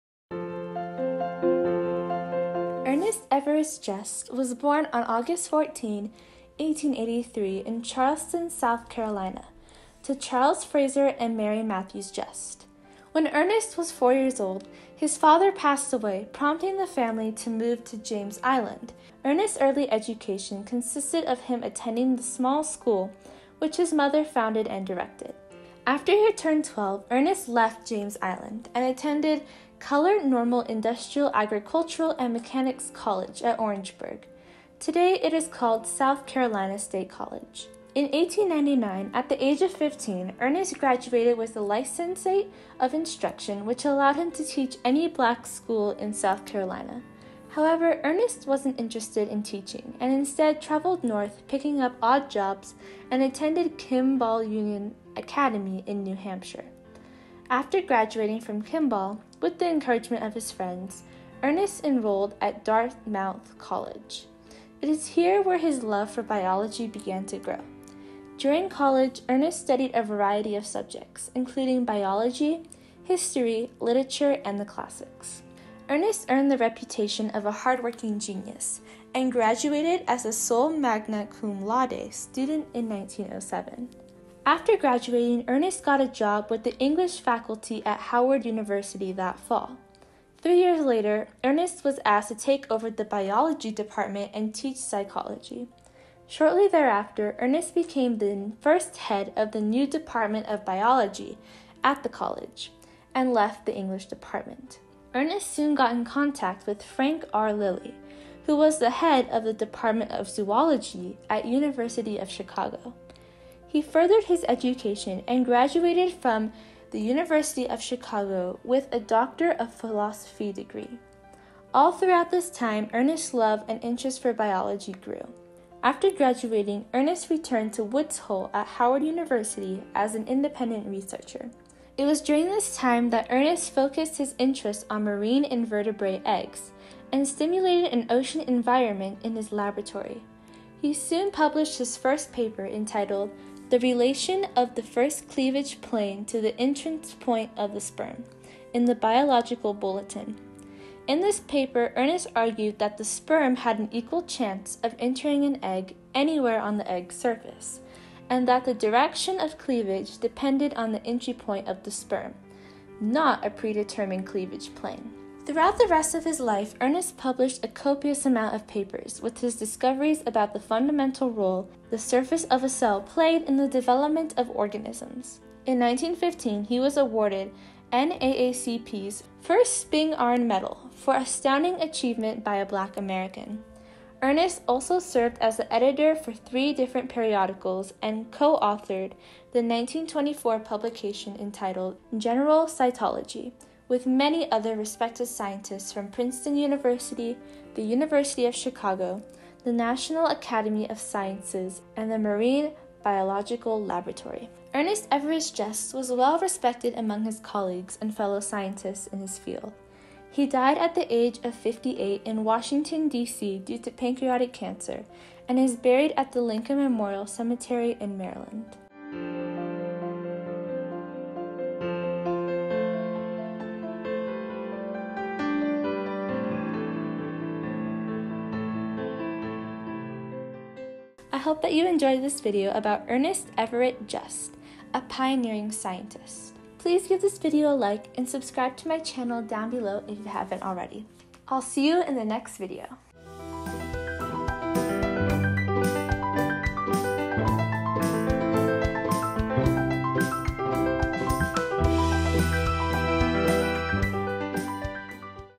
Ernest Everett Just | A Pioneering Scientist
What an amazing man! I hope that you enjoyed this short video!

Some pictures in this video were borrowed from different websites. Those websites, as well as the timestamp of the picture, are listed below. The remaining pictures were taken from free, commercial use sources such as Pixabay.

Picture Credits:
0:00 and 3:24
0:25
0:33
1:44
2:20
2:30
2:55
3:11
3:29